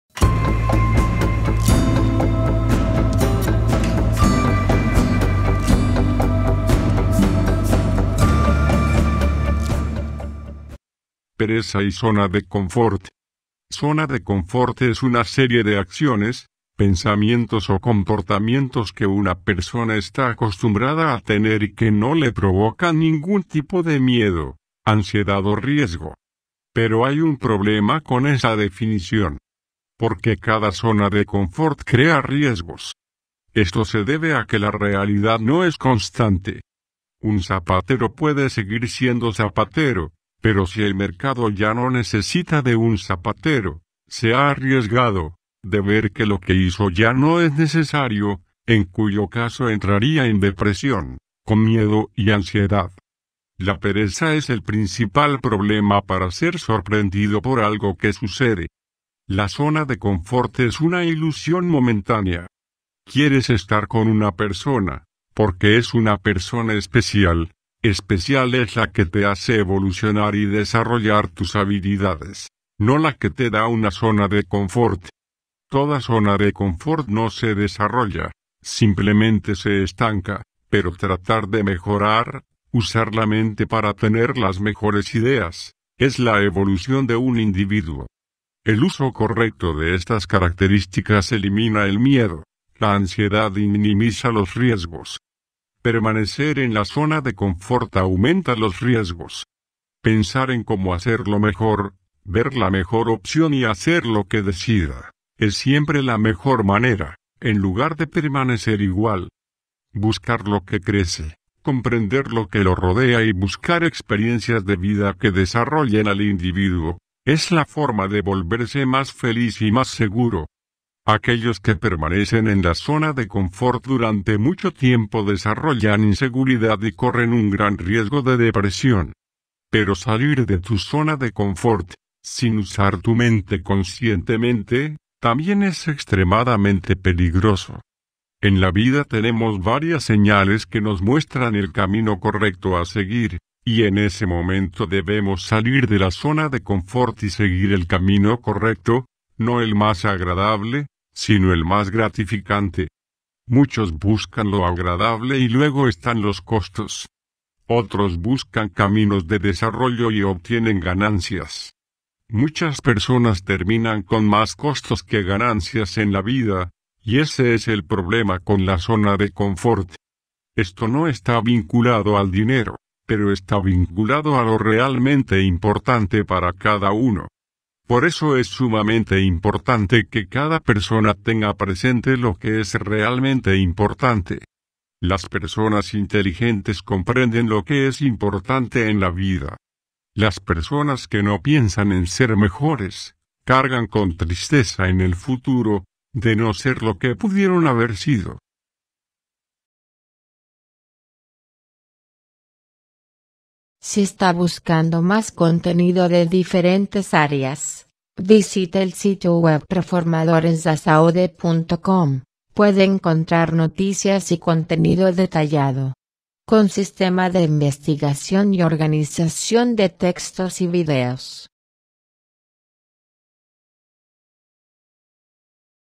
Pereza y zona de confort.
Zona de confort es una serie de acciones, pensamientos o comportamientos que una persona está acostumbrada a tener y que no le provocan ningún tipo de miedo, ansiedad o riesgo. Pero hay un problema con esa definición. Porque cada zona de confort crea riesgos. Esto se debe a que la realidad no es constante. Un zapatero puede seguir siendo zapatero, pero si el mercado ya no necesita de un zapatero, se ha arriesgado, de ver que lo que hizo ya no es necesario, en cuyo caso entraría en depresión, con miedo y ansiedad.
La pereza es el principal problema para ser sorprendido por algo que sucede. La zona de confort es una ilusión momentánea.
Quieres estar con una persona, porque es una persona especial, especial es la que te hace evolucionar y desarrollar tus habilidades, no la que te da una zona de confort. Toda zona de confort no se desarrolla, simplemente se estanca, pero tratar de mejorar, usar la mente para tener las mejores ideas, es la evolución de un individuo. El uso correcto de estas características elimina el miedo, la ansiedad y minimiza los riesgos. Permanecer en la zona de confort aumenta los riesgos.
Pensar en cómo hacer lo mejor, ver la mejor opción y hacer lo que decida, es siempre la mejor manera, en lugar de permanecer igual. Buscar lo que crece, comprender lo que lo rodea y buscar experiencias de vida que desarrollen al individuo, es la forma de volverse más feliz y más seguro.